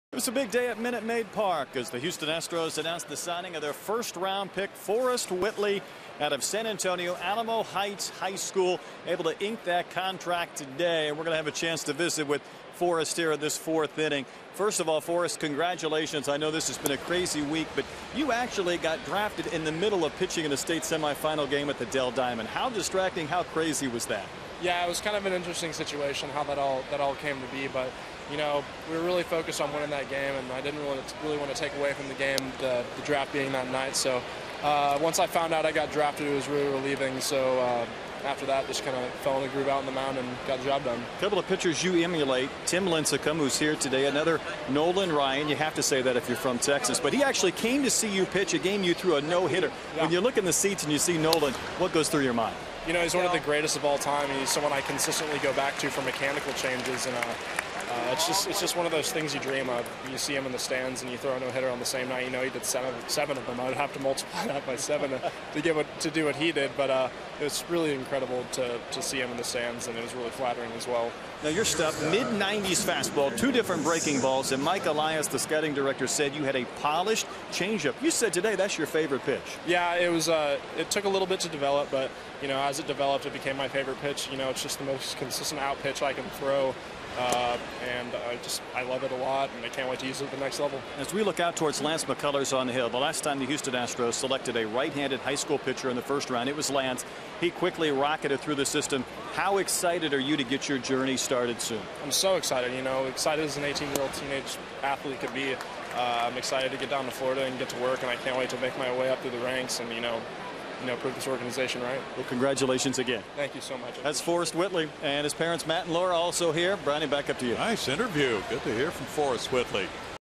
LAA@HOU: Whitley discusses Draft, pitching abilities
Astros first-round Draft pick Forrest Whitley discusses his strengths on the mound as well as how he found out he was drafted